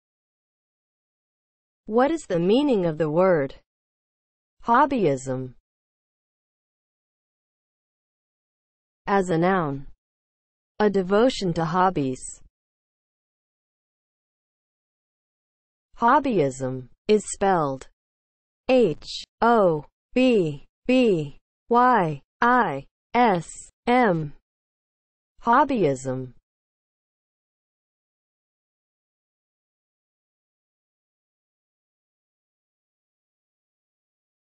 What is the meaning of the word HOBBYISM?
Definition and spelling of the word HOBBYISM.

(00:00) Intro
(00:08) As a noun
(00:14) Spelling

As a noun (00:08):
• a devotion to hobbies